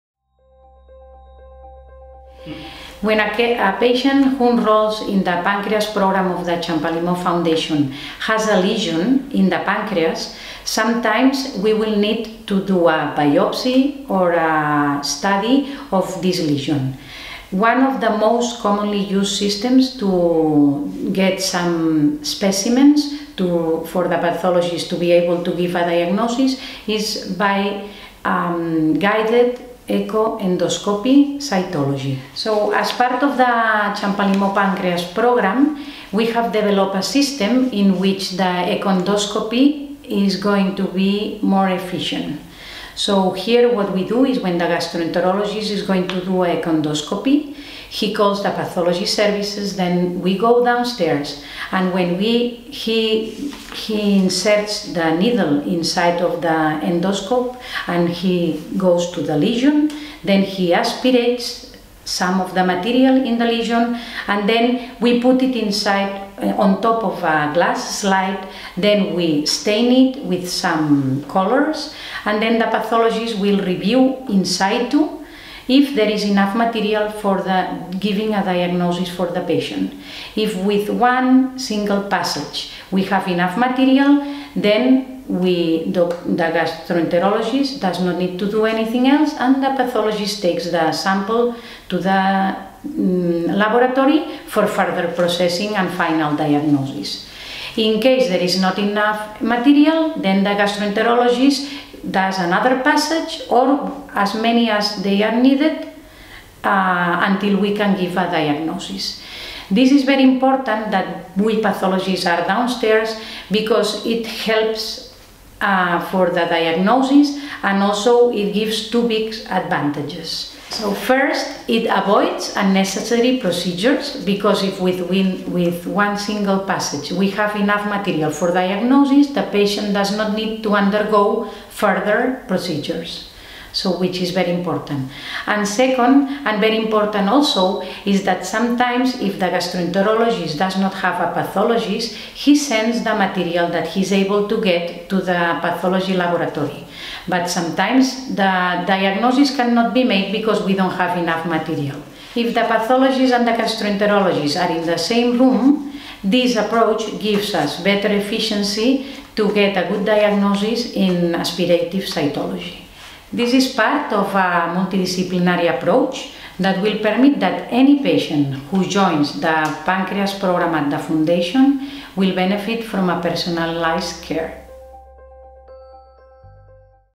Mireia Castillo - Champalimaud Pancreas Programme - Echo-Endoscopy
Dr. Castillo is a Researcher and Pathologist, and part of the Champalimaud Pancreas Fast-Track Programme. In this video, she reveals the novel role that pathology plays within echo-endoscopy.

SUBTITLES AVAILABLE IN PORTUGUESE / LEGENDAS DISPONÌVEIS EM PORTUGUÊS

Mireia Castillo  - Programa Champalimaud de Pâncreas – Ecoendoscopia
A Dra. Castillo é Investigadora e Patologista na Fundação Champalimaud, e parte do Programa Champalimaud de Pâncreas. Neste vídeo, ela revela o papel que a patologia desempenha na ecoendoscopia.

LEGENDAS DISPONÌVEIS EM PORTUGUÊS